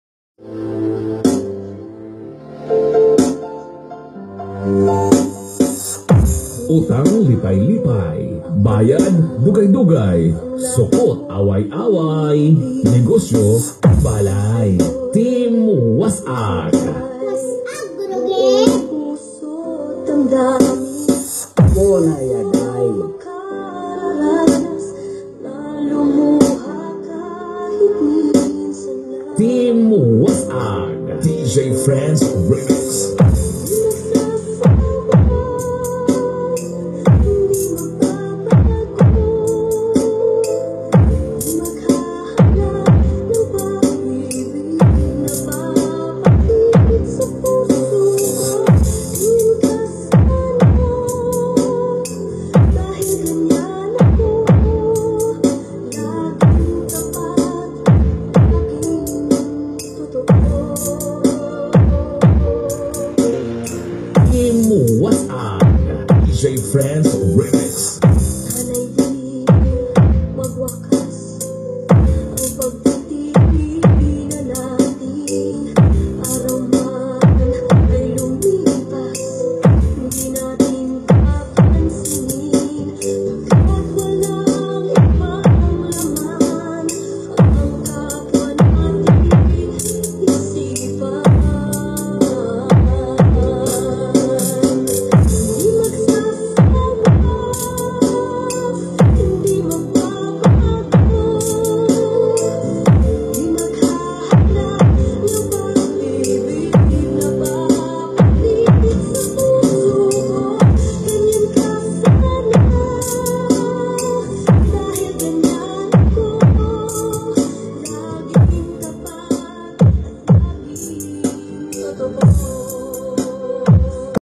Mini Dapulaza Sound System of Roxas City
Source: Facebook.
Cerwin Vega Earthquake Box Subwoofer Design.
Bullet Mid/Hi Speakers.